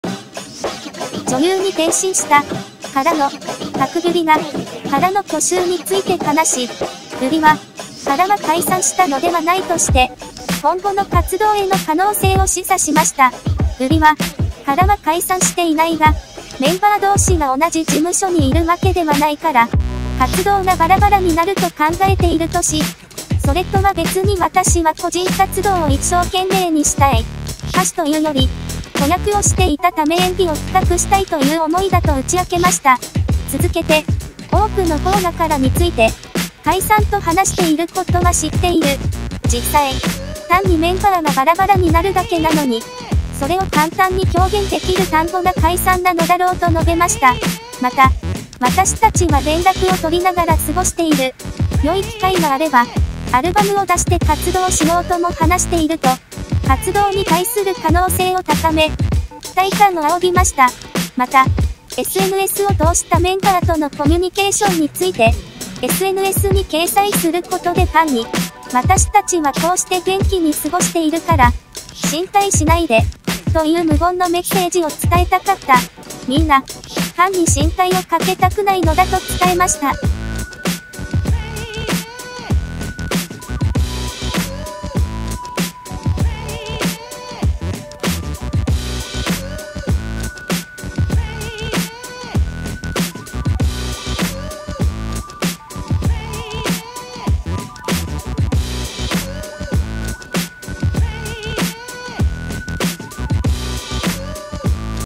女優 KARAのパク・ギュリ、解散を否定マイ ムービー
女優に転身した、からの　パクギュリが、からの去就について話し、
ギュリは、からは解散したのではないとして、今後の活動への可能性を示唆しました。